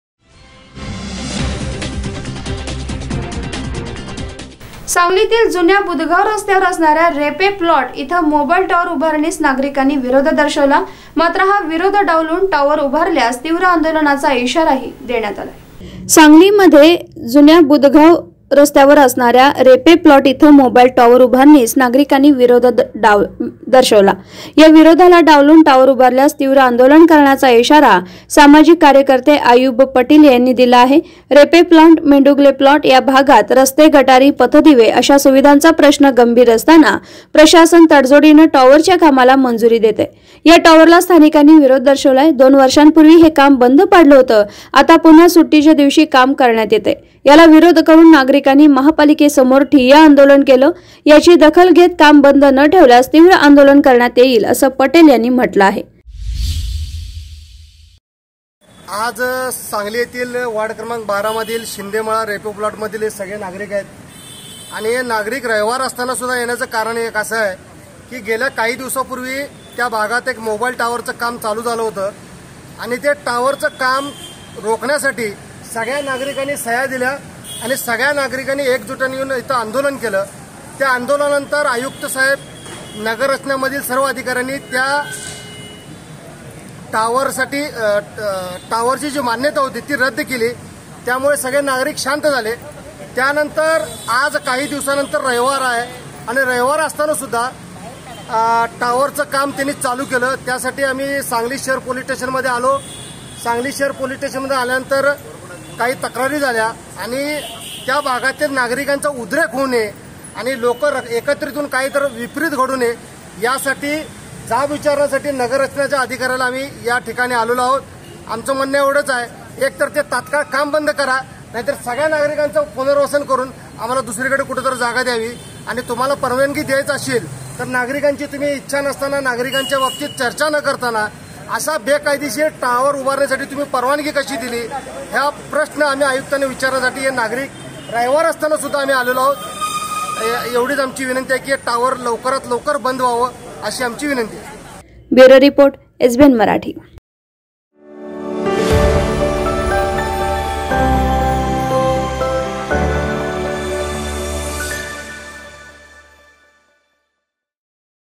SANGLI | सांगलीतील मोबाइल टॉवर उभारणीस नागरिकांचा विरोध
सांगलीतील जुन्या बुधगाव रस्त्यावर असणाऱ्या रेपे प्लॉट येथे मोबाइल टॉवर उभारणीस नागरिकांचा विरोध, विरोध डावलून टॉवर उभारल्यास तीव्र आंदोलनाचा इशारा